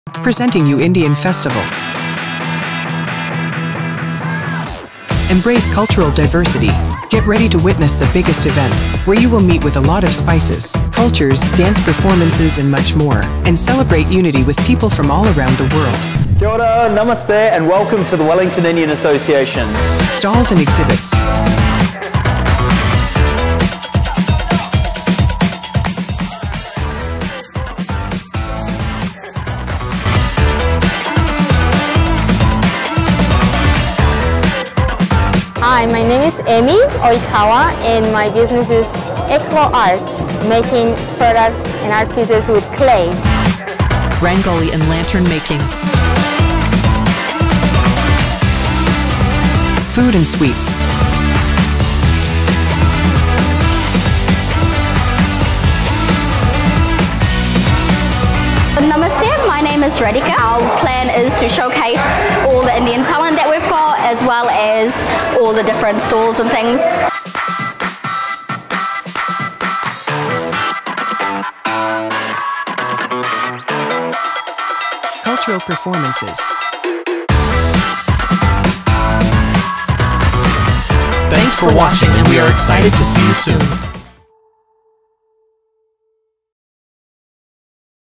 Namaste Wellington 2022
Get excited for another sneak peek of the upcoming Namaste Wellington 2022 event! Last year's celebration was incredible, and we're gearing up for more global unity and diversity this weekend.

expertly filmed by RuchikaJainPhotography and edited by HuttCreativeStudio